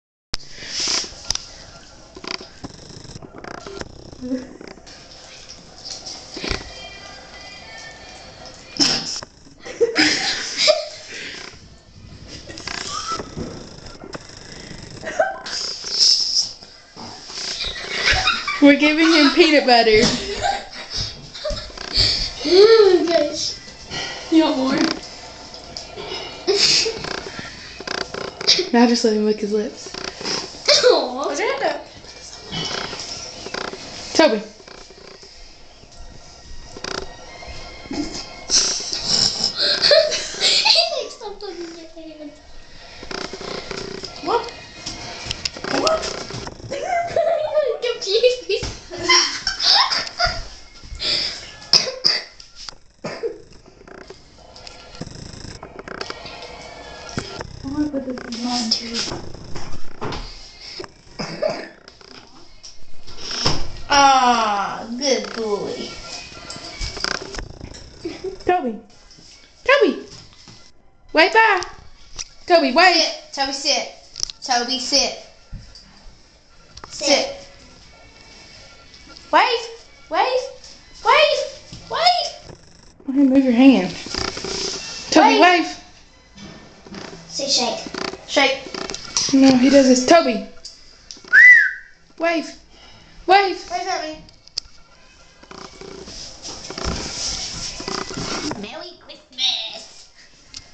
Toby eating Peanut Butter
This is my black miniature poodle Toby, he is soooo cute and i love him!!! This is him eating peanut butter!!!